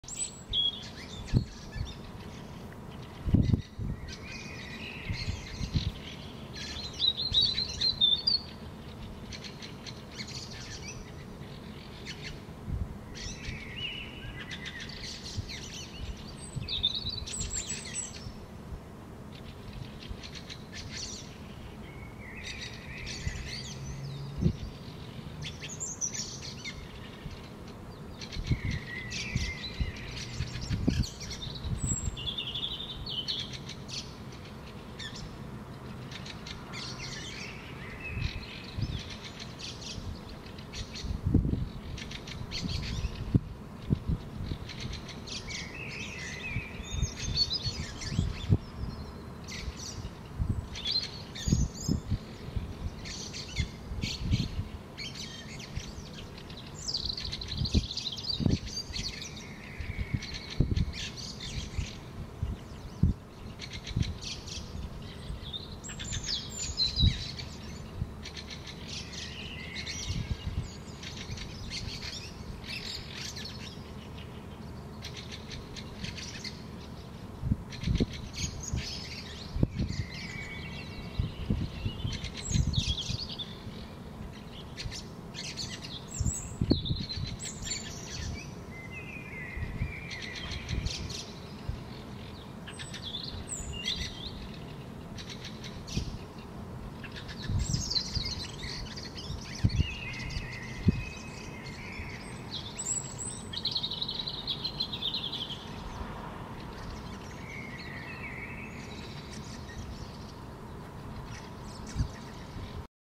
Waning gibbous Moon rising from behind the rooftops
Sounds of European Robin (Erithacus rubecula), Fieldfare (Turdus pilaris) and Common Blackbird (Turdus merula) can be heard in the background.

Vähenevä kupera Kuu nousee kattojen takaa. Taustalla kuuluu punarinnan (Erithacus rubecula), räkättirastaan (Turdus pilaris) ja mustarastaan (Turdus merula) ääniä.